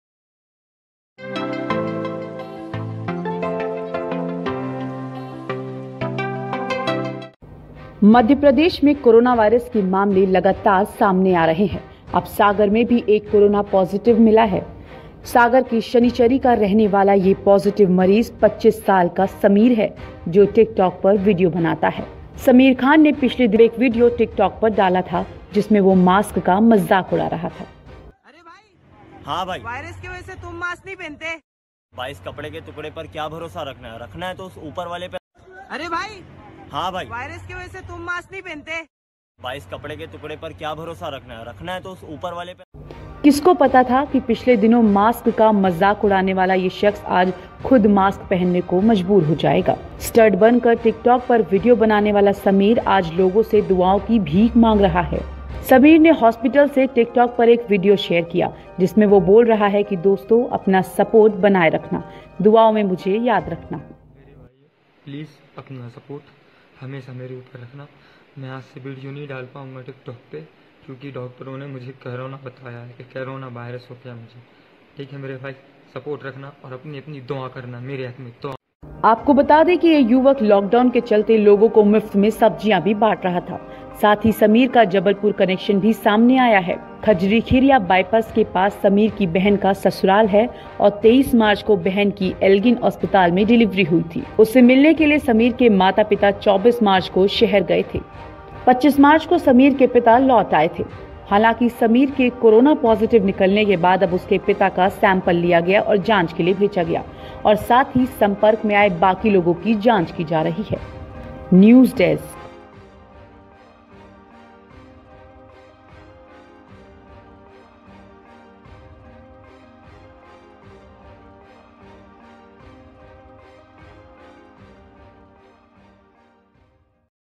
Sagar: TikTok पर मास्क का मज़ाक उड़ाने वाला Corona Positive निकला, अब मांग रहा दुआओं की भीख
मध्य प्रदेश में कोरोना वायरस के लगातार मामले सामने आ रहे हैं. अब सागर में भी एक कोरोना पॉजिटिव मरीज मिला है. सागर के शनि-चरी का रहने वाला ये पॉजिटिव मरीज 25 साल का समीर है जो टिकटॉक पर वीडियो बनाता है। समीर खान ने पिछले दिनों जब कोरोना वायरस के आंकड़े लगातार बढ़ रहे थे तब एक टिकटॉक वीडियों डाला था जिसमें वो मास्क का मजाक उड़ा रहा है।